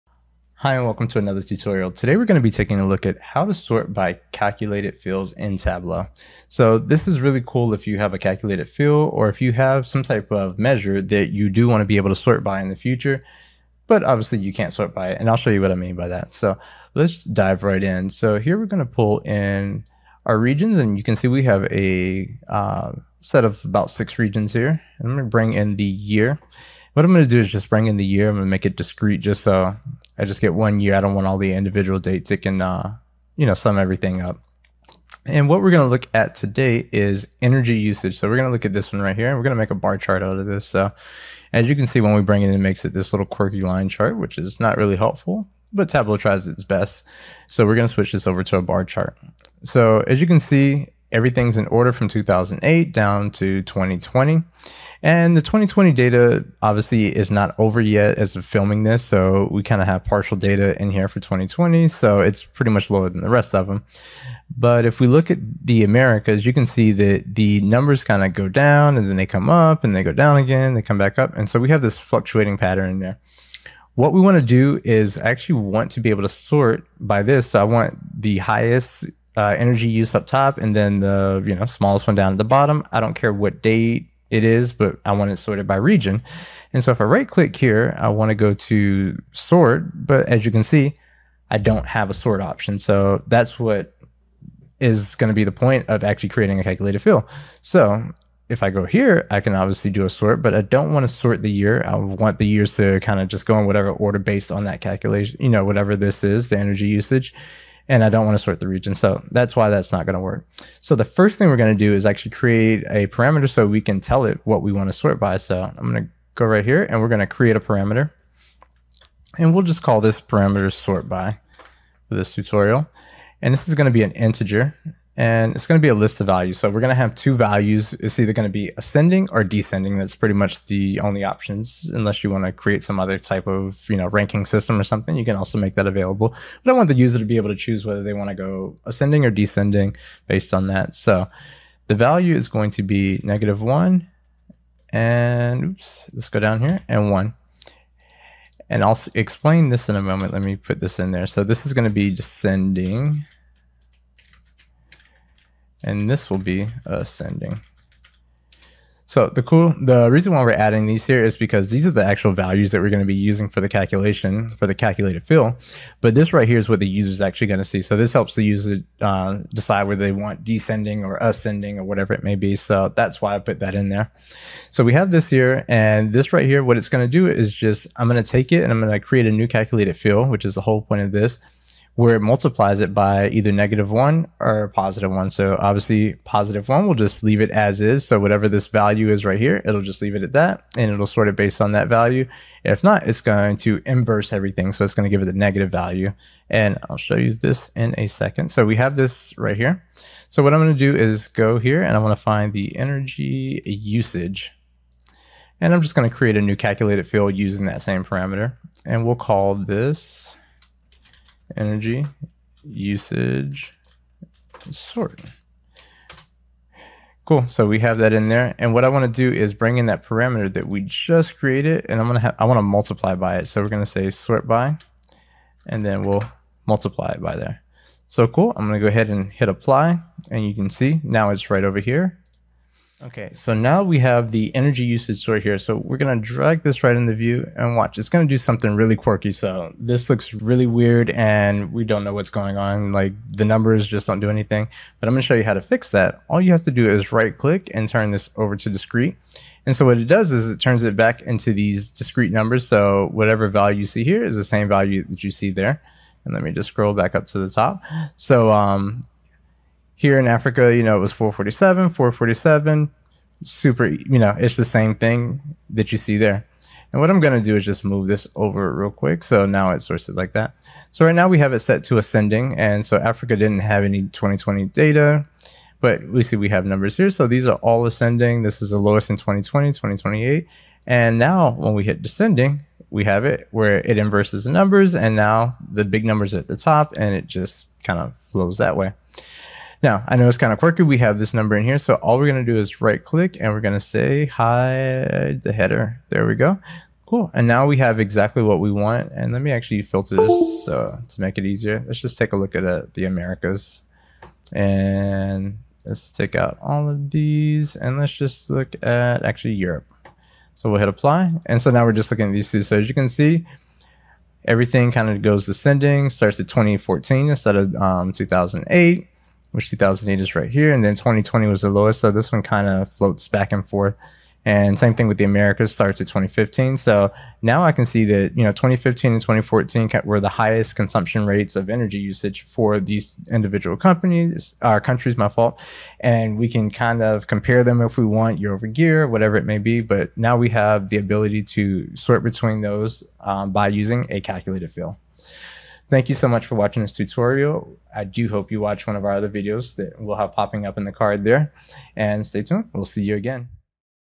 Tableau - How to Sort by Calculated Field
In this tutorial, we will show you how to sort by a calculated field in Tableau. This will allow you to sort by a measure and not only by a dimension which is the way Tableau works out of the box. We will also show you how to create a bar chart in our example even when Tableau trys to make it into a line chart.

Do you want to become a Tableau Master?